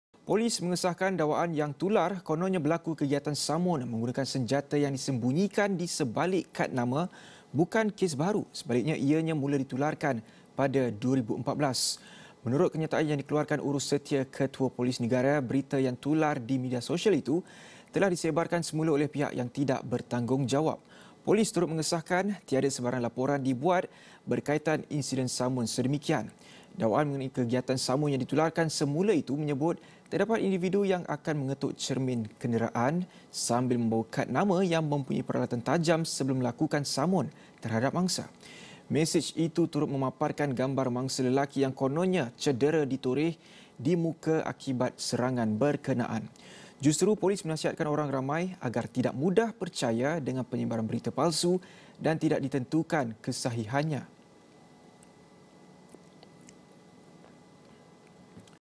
Berita Palsu | Insiden samun guna kad bukan kes baharu
Polis mengesahkan dakwaan yang tular kononnya berlaku kegiatan samun menggunakan senjata yang disembunyikan di sebalik kad nama, bukan kes baharu, sebaliknya ianya mula ditularkan pada 2014.